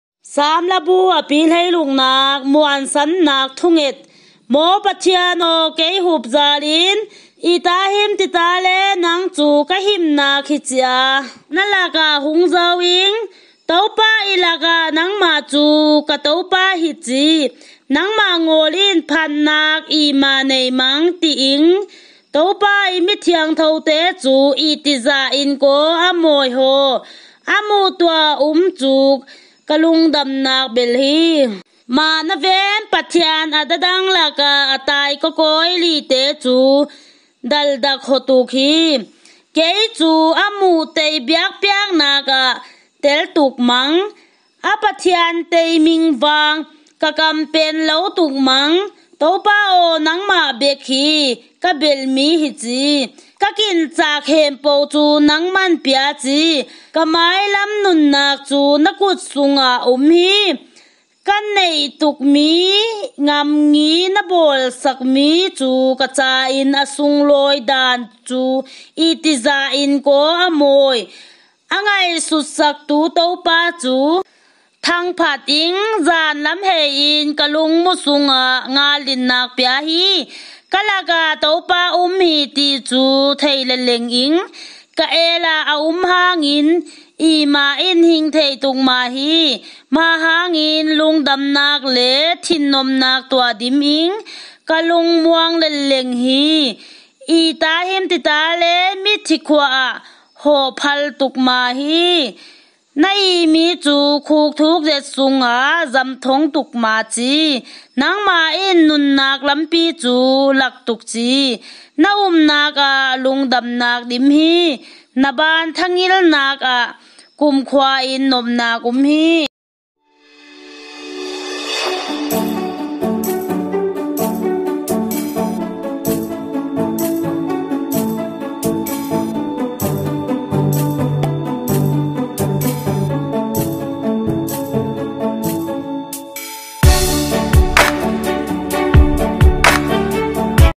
SAAM LABU - 16 MUANSAN NAK THU NGET ( NGAWN BIBLE ) 🌐✝️🌐
SAAM LABU - 16

Muansannak Thungeta

16

1 Maw Pathian aw, kei hup zalin; itahim titale nang cu ka himnak hi ci a, na lakah hung zo ing.

2 Topa i lakah, “Nangmah cu ka Topa hi ci, Nangmah ngawlin phatnak imah nei mang,” ti ing.

3 Topa i mi thiangthote cu itih zahin kaw a mawi haw! Amuh tuah um cu ka lungdamnakbel hi.

4 Mahnaven pathian dadang lakah a tai kawkawi lite cu daldak haw tuk hi. Kei cu amuh tei biakpiaknakah tel tuk mang; A pathian tei min vang ka kam pen lo tuk mang.

5 Topa aw, nangmah bek hi ka belhmi hi ci; Ka kincak hempoh cu nangman pia ci. Ka mailam nunnak cu na khut sungah um hi.

6 Ka neih tukmi ngamngi na bawlsakmi cu ka ca in, a sunglawi daan cu itih zahin kaw a mawi?

7 A ngaihsut saktu Topa cu thangphat ing;
Zanlam hei in ka lungmu sungah ngallinnak pia hi.

8 Ka lakah Topa um hi tih cu thei leleng ing;
Ka elah a um hangin, imah in hing thei tukma hi.

9 Mah hangin lungdamnak le thinnopnak tuah dim ing; Ka lungmuang leleng hi.

10 Itahim titale mithi khua ah hawh phal tukma hi, Na ihmi cu khuk thukzet sungah zamthong tukma ci.

11 Nangmah in nunnak lampi cu lak tuk ci; Na umnakah lungdamnak dim hi. Na baan tha ngilnakah kumkhua nopnak um hi.